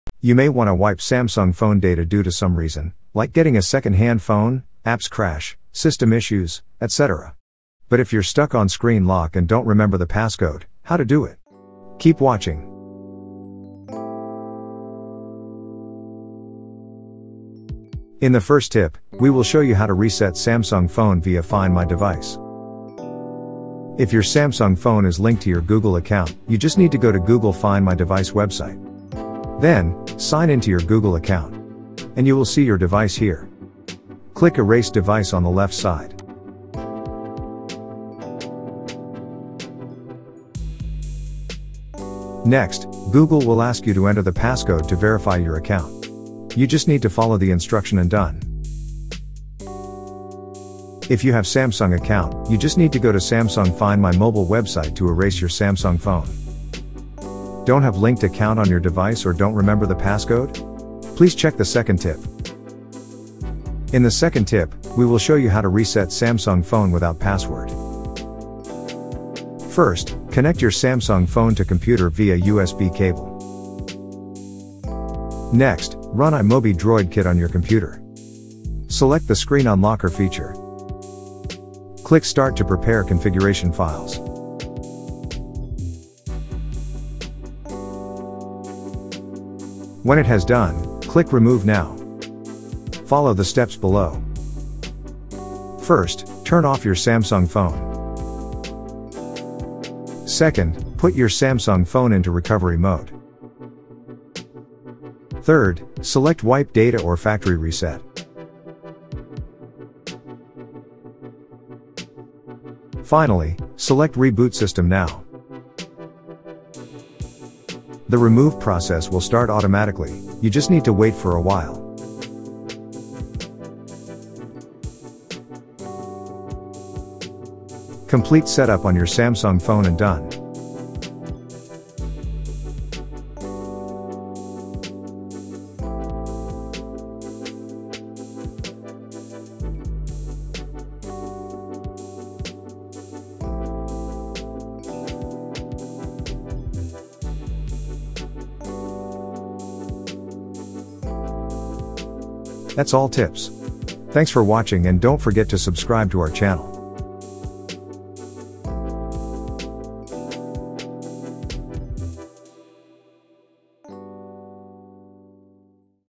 [2 Ways] How to Reset Samsung Phone without Password 2022
if you’re stuck on screen lock and don’t remember the passcode but wanna wipe your Samsung, please don't miss the video!

You may wanna wipe Samsung phone data due to some reason, like getting a second-hand phone, apps crash, system issues, etc. But if you forgot the screen password, how to reset Samsung phone without password? Keep watching!
00:00 Intro
00:18 Tip 1. Reset Samsung phone via Find My Device
01:05 Tip 2. Reset Samsung phone without password